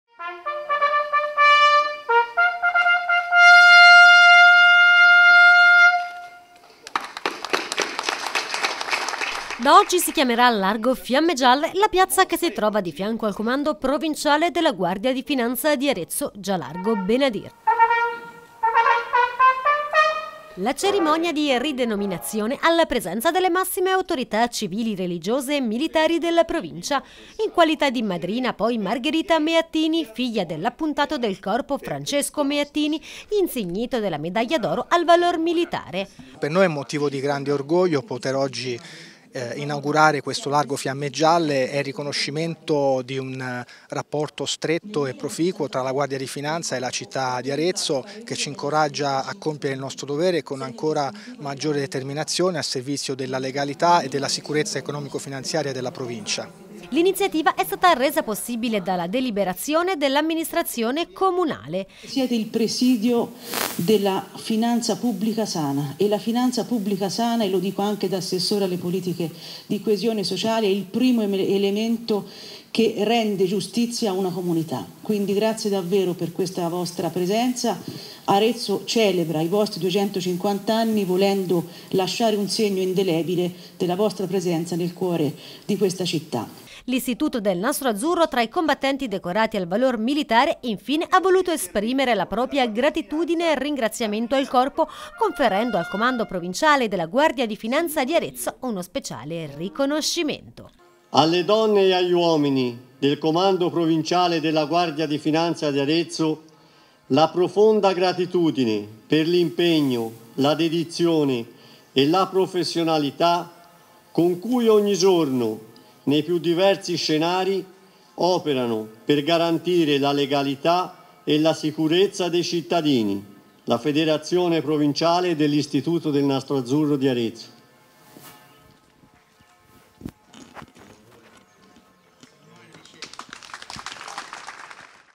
250 anni della Guardia di Finanza, Ad Arezzo nasce "Largo Fiamme Gialle"
Da oggi si chiamerà “Largo Fiamme Gialle” la piazza che si trova di fianco al Comando provinciale della Guardia di Finanza di Arezzo, già Largo Benadir.